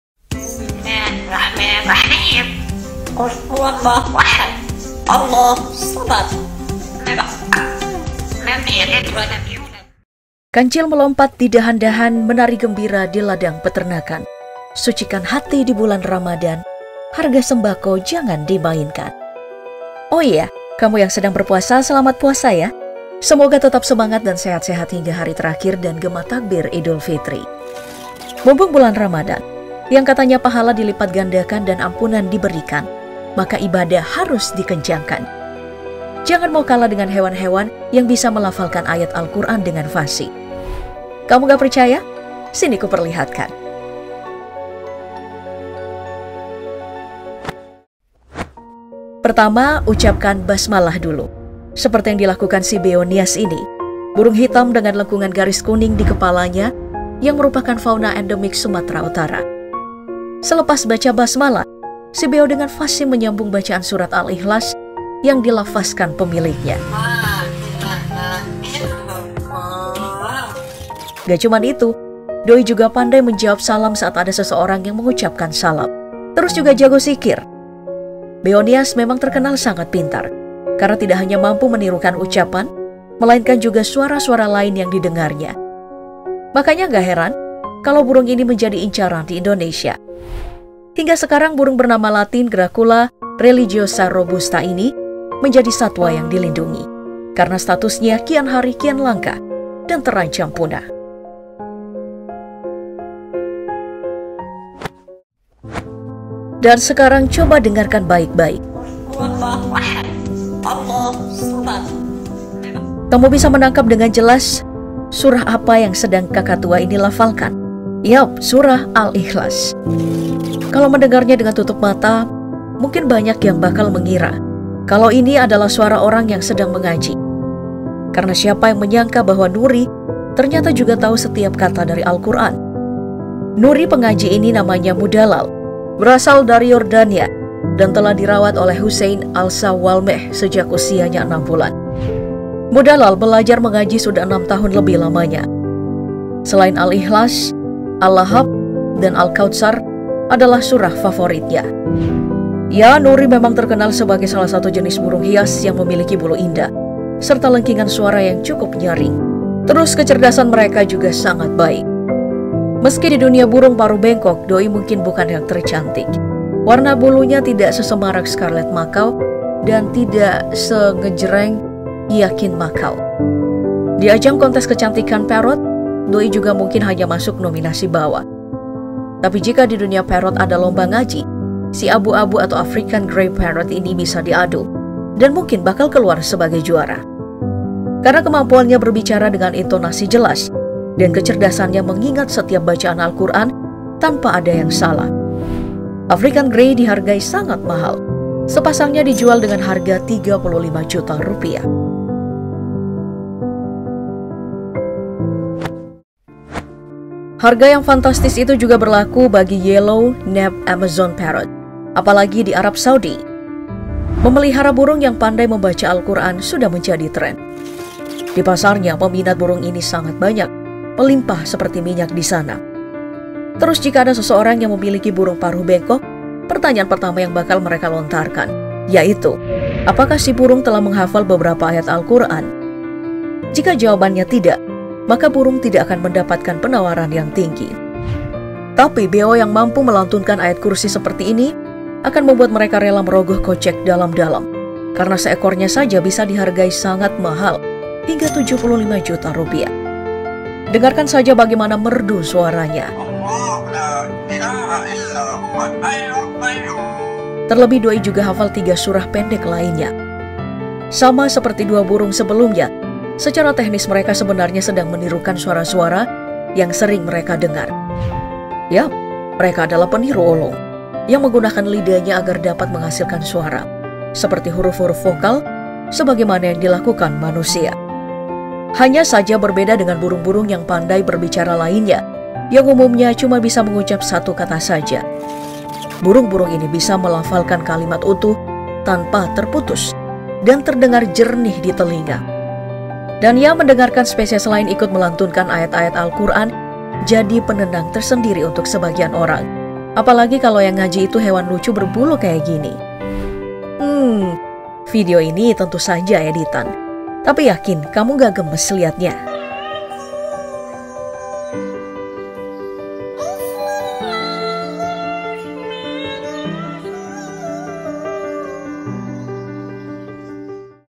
MASYA ALLAH!!! Orang² Tercengang Saat Mendengar Kucing ini Mengaji
Kancil melompat di dahan-dahan. Menari gembira di ladang peternakan. Sucikan hati di bulan ramadhan. Harga sembako jangan dimainkan. Ehiya, kamu yang sedang berpuasa, selamat puasa yah. Semoga tetap semangat dan sehat-sehat hingga hari terakhir dan gema takbir Idul Fitri.

Mumpung bulan ramadhan, yang katanya pahala dilipatgandakan dan ampunan diberikan, maka ibadah harus dikencangkan. Jangan mau kalah dengan hewan-hewan yang bisa melafalkan ayat alquran dengan fasih. Kamu ga percaya? sini kuperlihatkan.